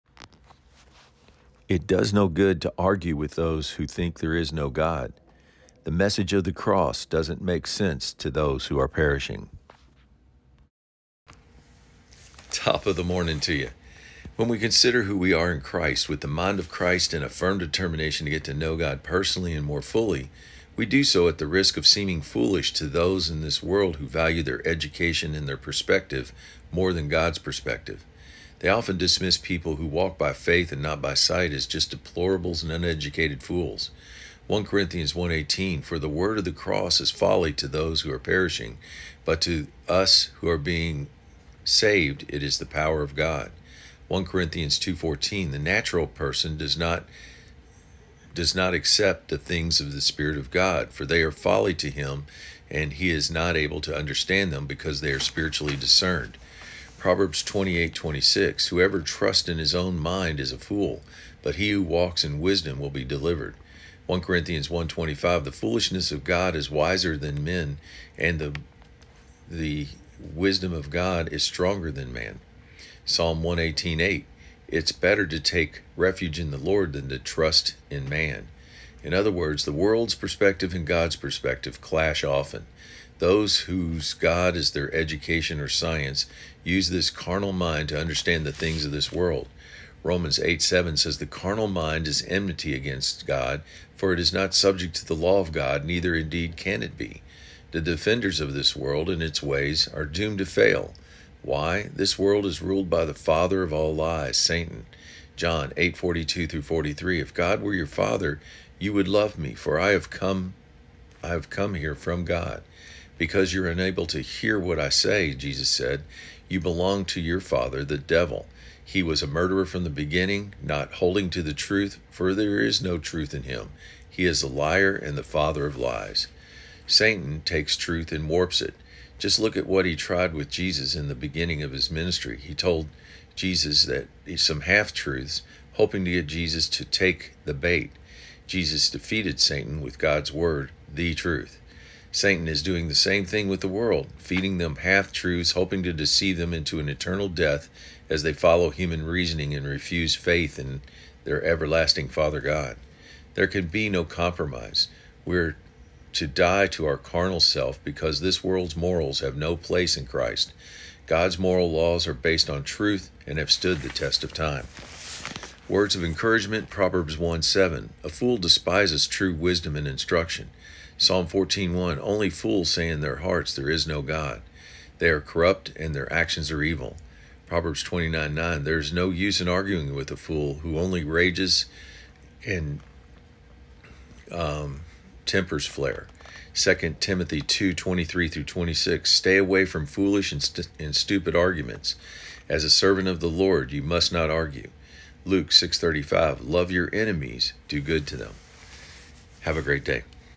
God arguments are futile!
Arguments about faith between believers and non-believers is futile because the things of God are spiritually discerned, unable to be figured out with the carnal mind.